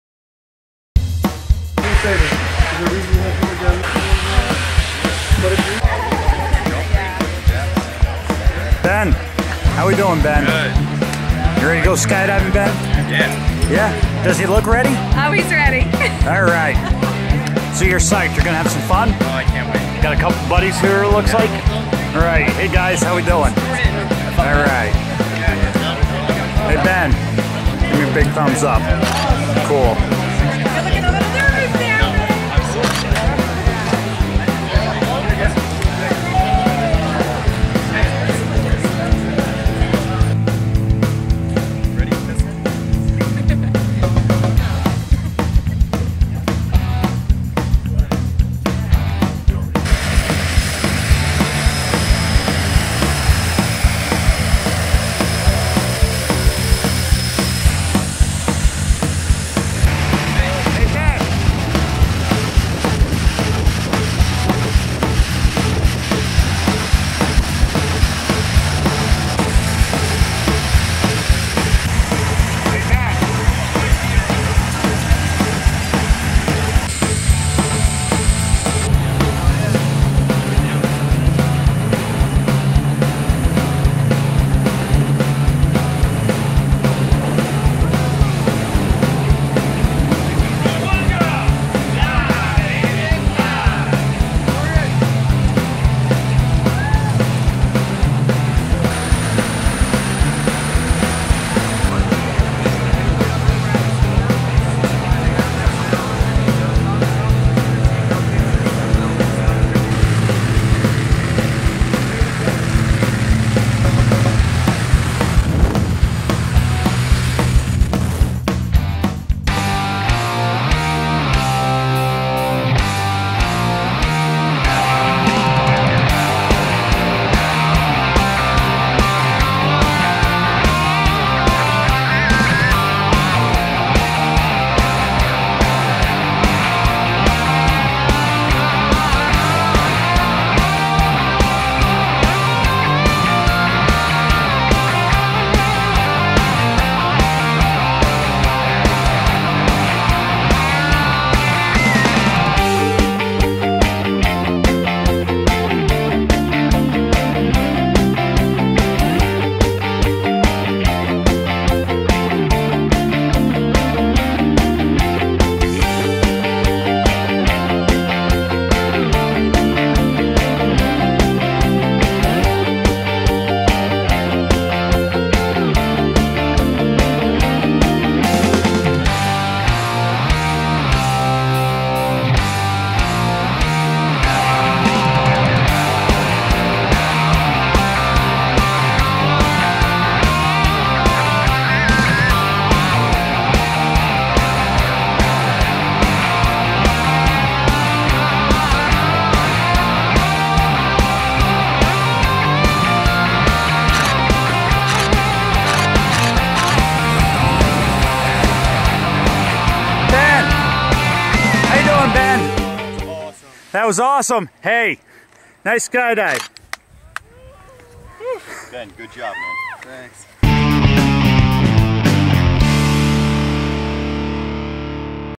First Jump Skydive
Skydive; July 31,2011 at Skydive The Ranch in NY
About a minute of freefall, and about 5 minutes of parachuting.
It was AMAZING, this place was great!